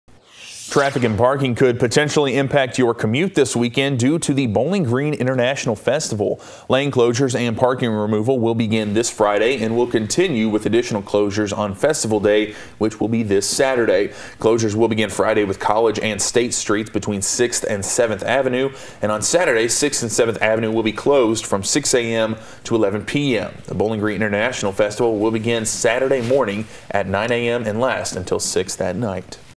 The Bowling Green International Festival could impact weekend commutes
Closures begin Friday with College and State Streets between 6th & 7th Avenue. Saturday, 6th & 7th Avenue will be closed from 6am - 11pm.